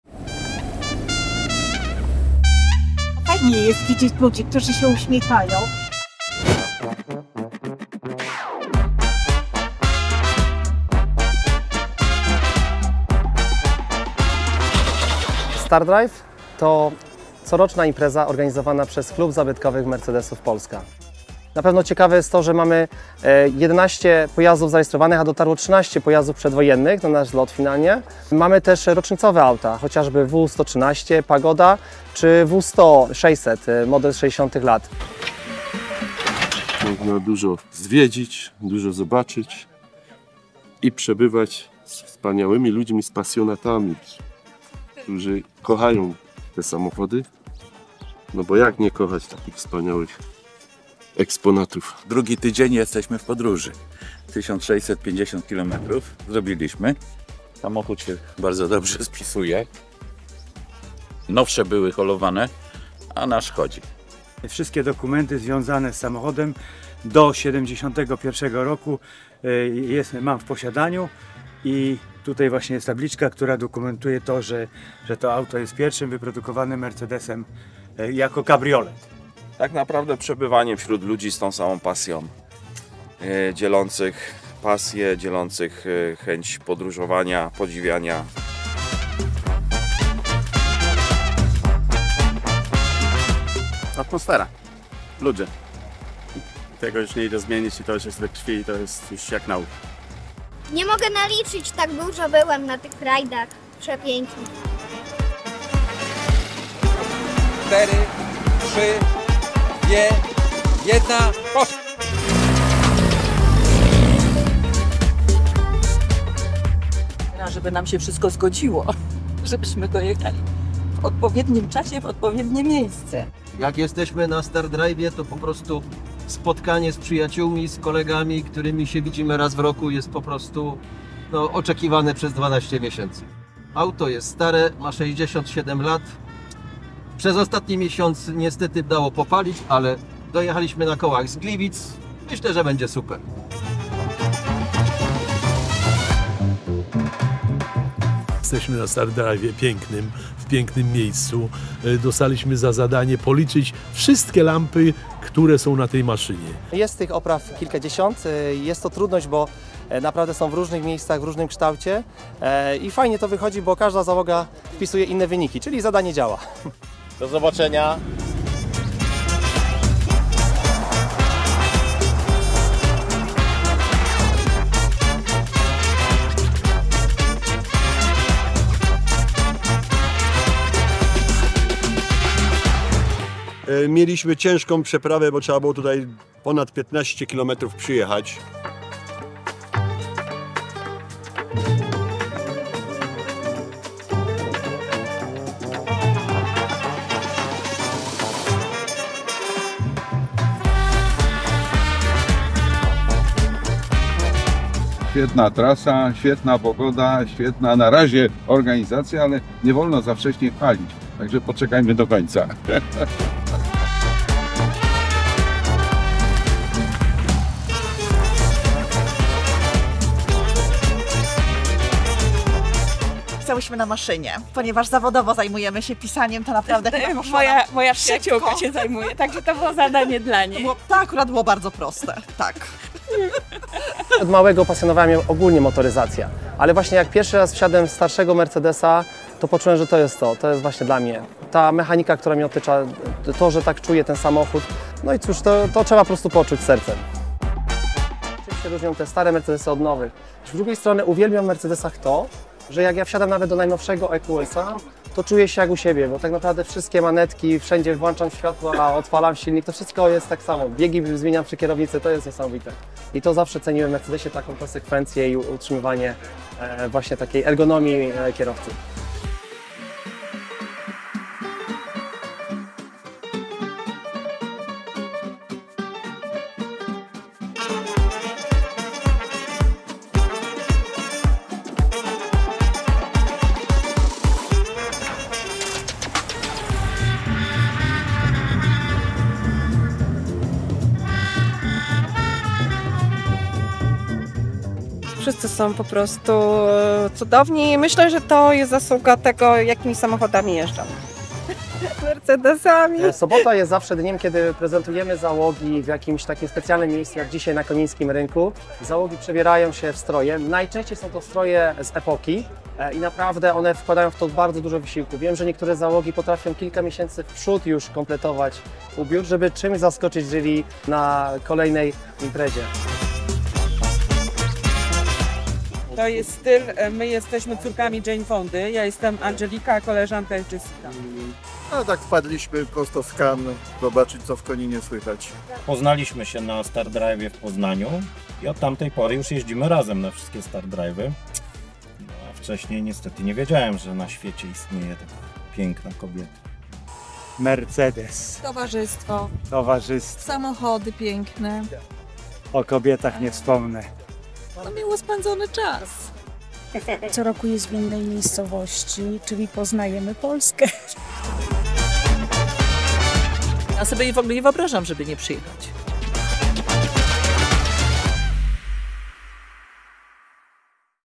Zlot historycznych samochodów Mercedes-Benz! Star Drive Poland 2023.
85 załóg z Polski i Europy. Pierwszy fabryczny kabriolet Mercedes-Benz. Słynny roadster Pagoda W 113 i limuzyna W 100, obchodzące w tym roku swoje 60 urodziny. Zobacz najpiękniejsze samochody Mercedes-Benz w Polsce i poznaj ich wspaniałych właścicieli w relacji ze zlotu Star Drive Poland 2023.